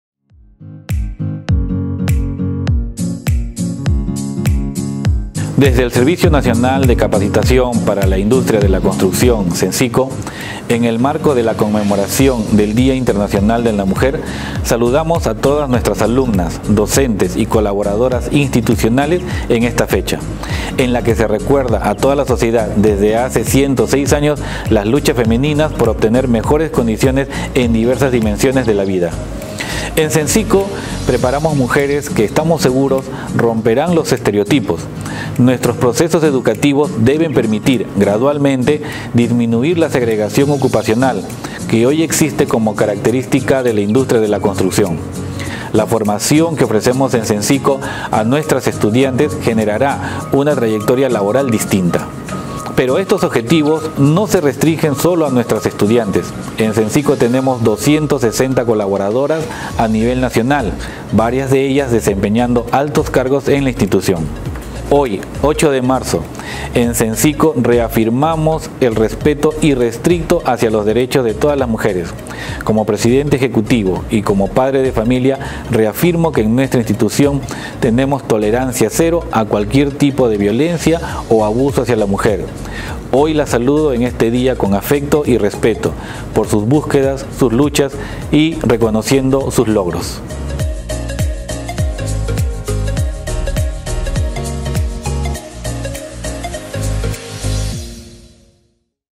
Sencico reafirma su respeto irrestricto hacia los derechos de todas las mujeres
Dr. Miguel Estrada, presidente ejecutivo de Sencico, envía un saludo a todas nuestras alumnas y colaboradoras ante la conmemoración del Día Internacional de la Mujer.

"En Sencico formamos mujeres que, estamos seguros, romperán los estereotipos", indica ✊🏼.